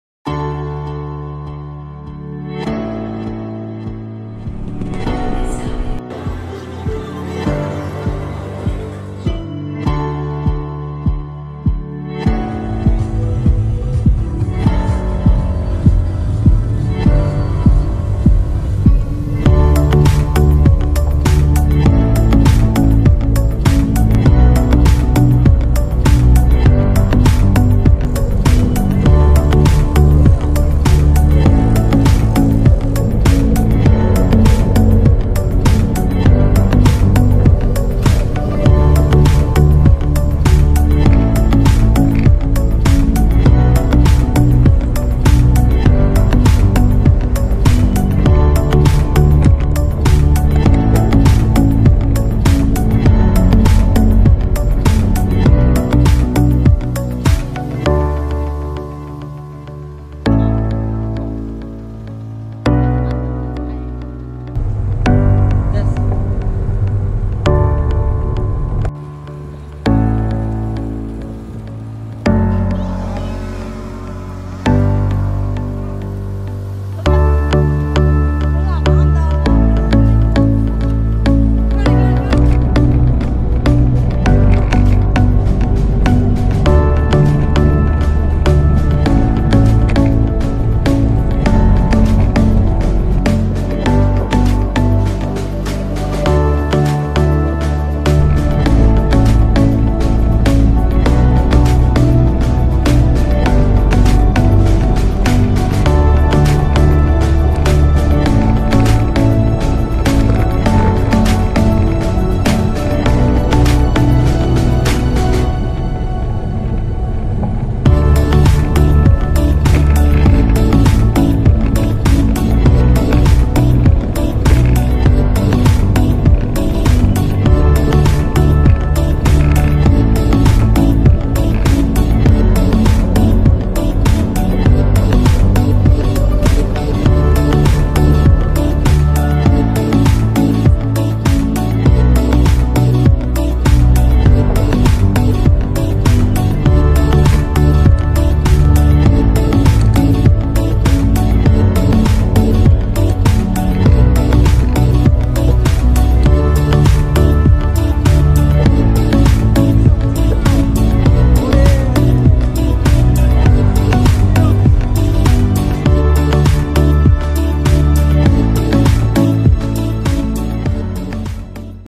2019 서울 자전거대행진 (2019.06.09)
세번째 참가하는 서울 자전거대행진에서의 영상 기록.